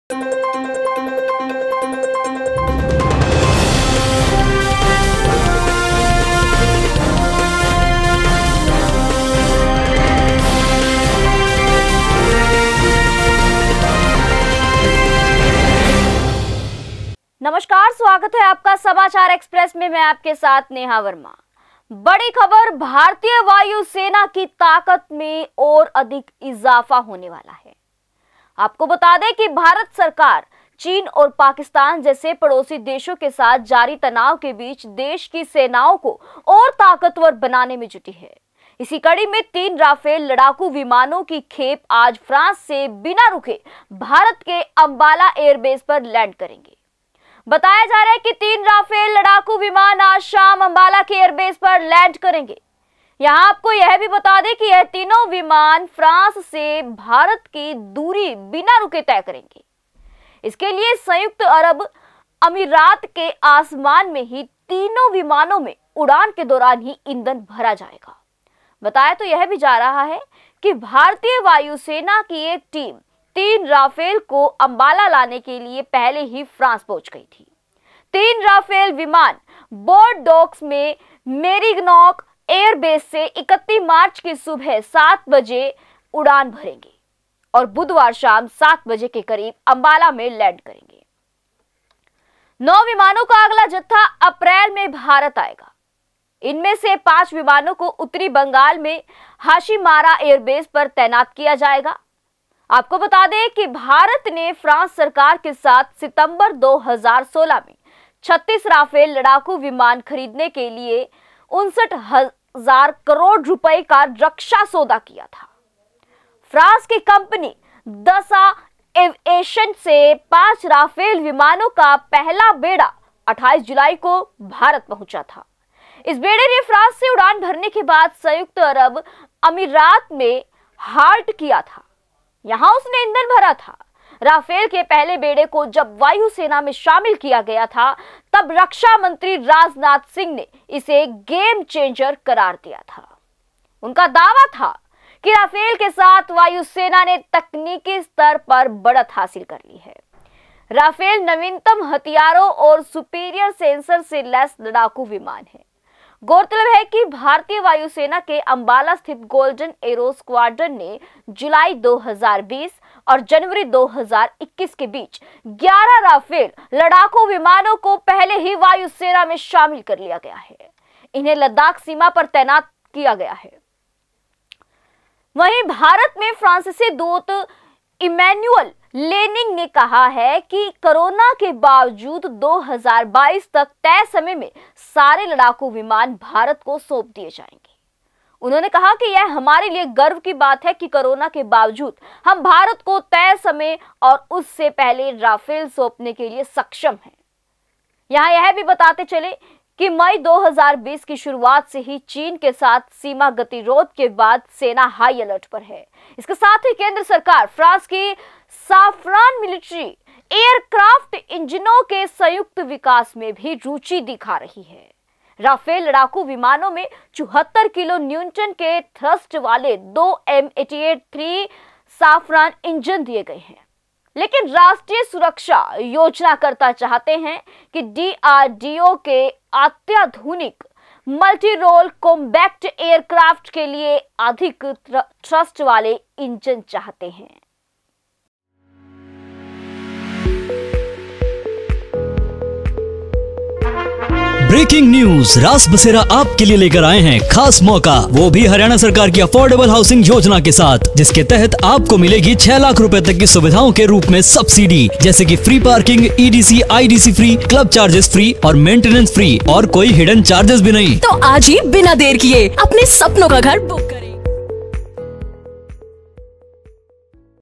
भारतीय वायुसेना की ताकत में होगा और अधिक इजाफा !
भारतीय वायुसेना की ताकत में और अधिक इजाफा होने वाला है। आपको बता दें कि भारत सरकार चीन और पाकिस्तान जैसे पड़ोसी देशों के साथ जारी तनाव के बीच देश की सेनाओं को और ताकतवर बनाने में जुटी है। इसी कड़ी में तीन राफेल लड़ाकू विमानों की खेप आज फ्रांस से बिना रुके भारत के अंबाला एयरबेस पर लैंड करेंगे। बताया जा रहा है कि तीन राफेल लड़ाकू विमान आज शाम अंबाला के एयरबेस पर लैंड करेंगे।